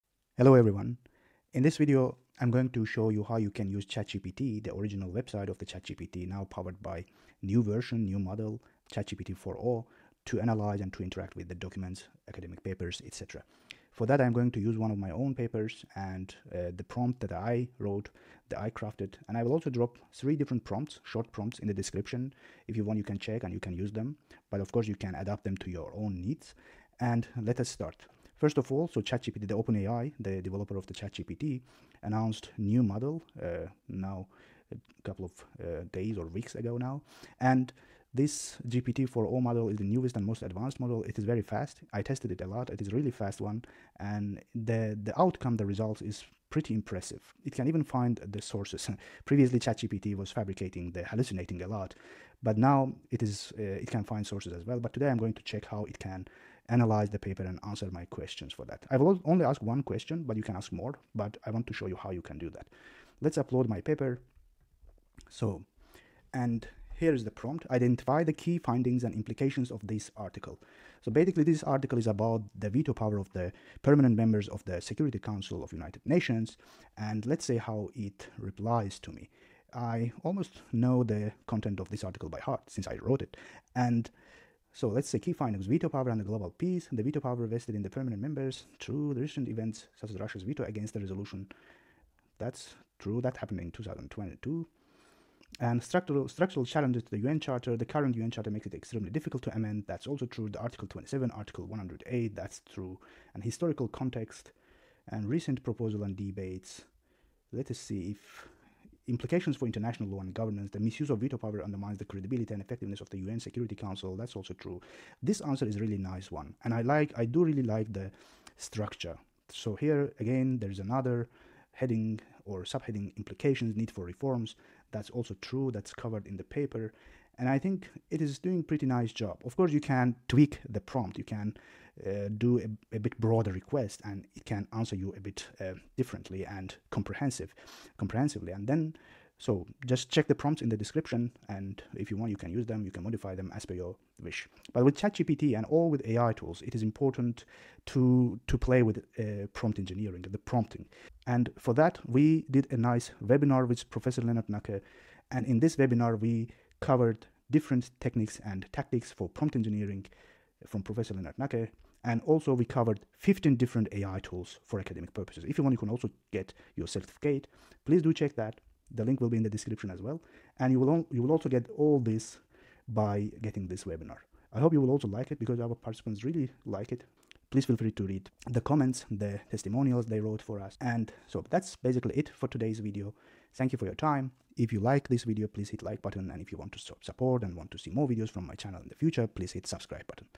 ChatGPT-4o To Analyse Academic Papers + 3 Useful Prompts
I tested ChatGPT-4o to analyse my own paper and here is the result.

3 Prompts for Summarising and Analysis Academic Sources:

1. Summarise the main arguments in this article/paper

2. Identify the key findings and implications of this research paper

3. Describe the theoretical framework of this article and how it relates to the findings

You can tweak these prompts as per your own needs.

With Prof. Lennart Nacke, we covered different strategies, tactics and prompt samples, as well as clear use cases of AI tools with live demos.